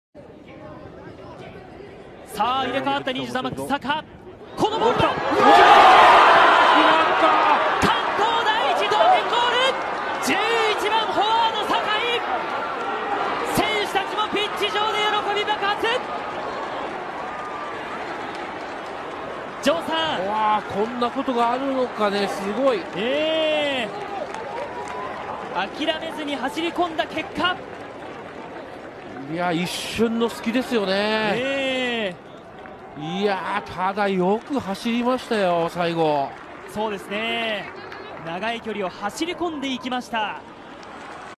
【王者撃沈】静岡学園を沈めたカウンターが強烈だった、、
サッカーは何が起こるかわからない、
まさに執念